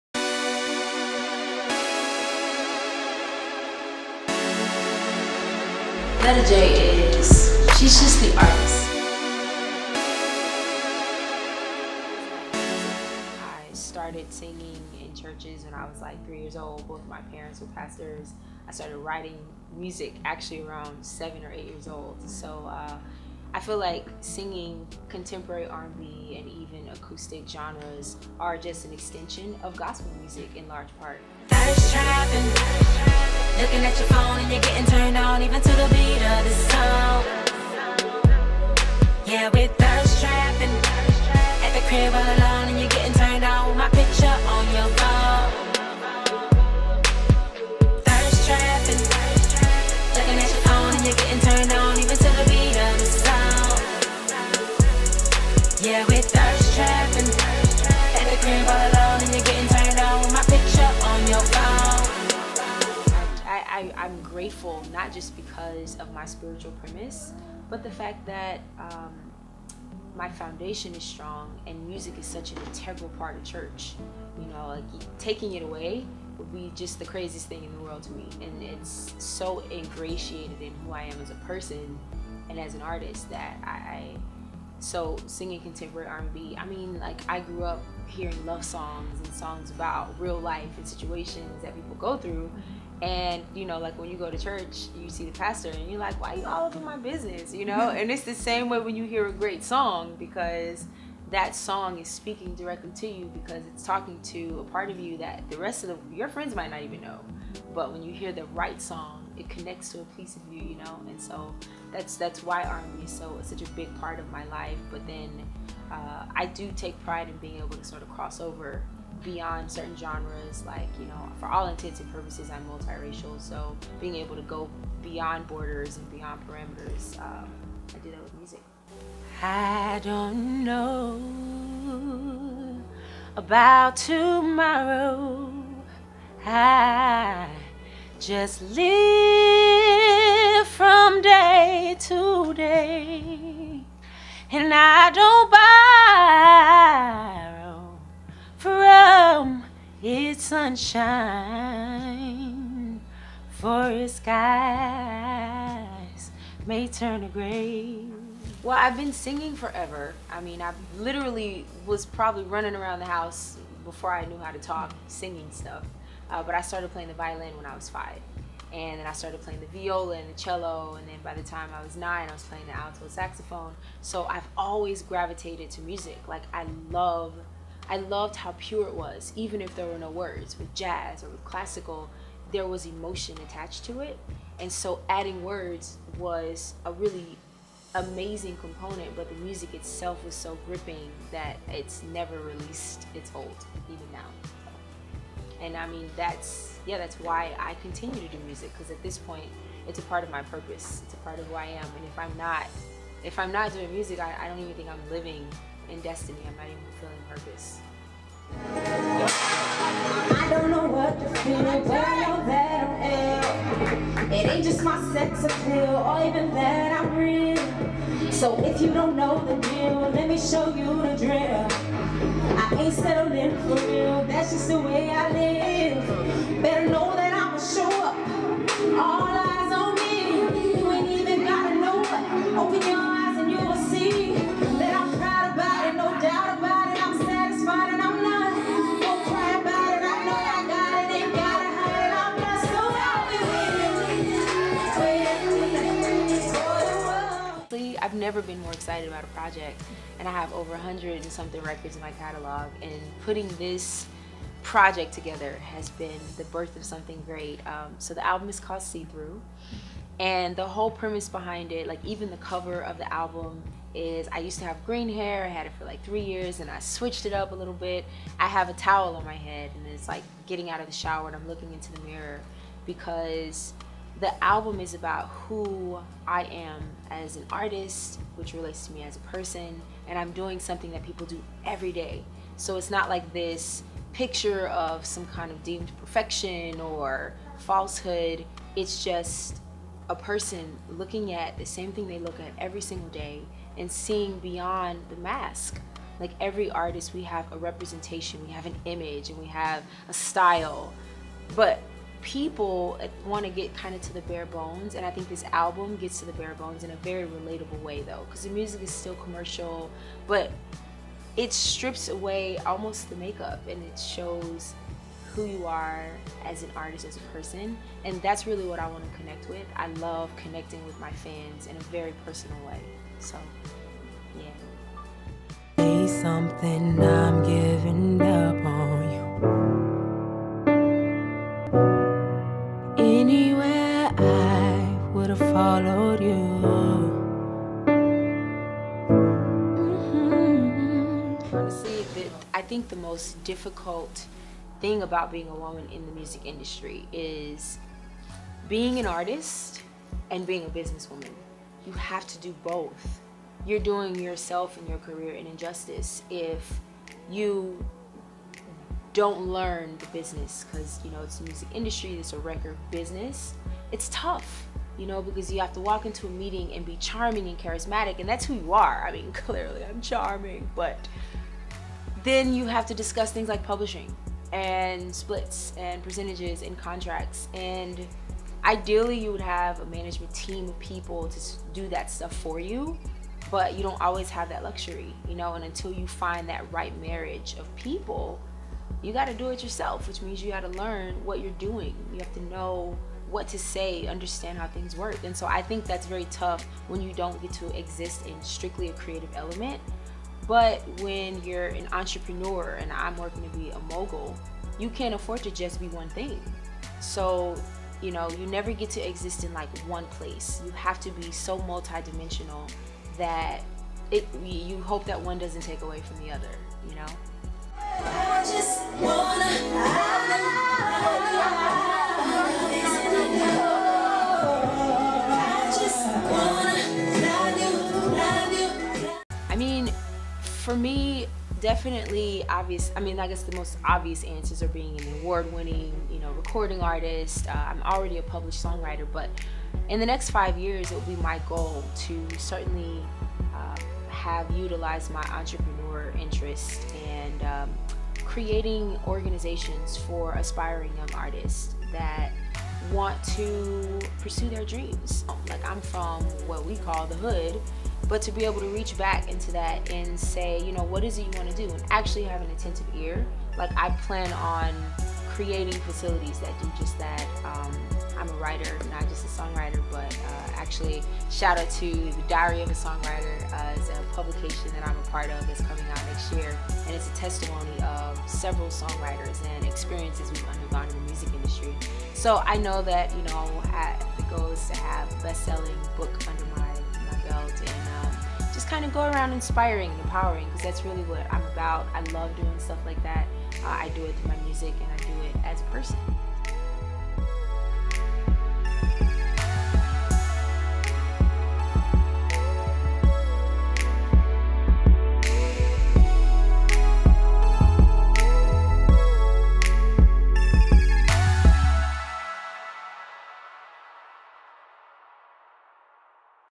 Letta J Southern Songstress "Video Interview"
Letta J Interviews with The Next Level Magazine Talks about Upcoming Album being a women in the music Industry And Future Plans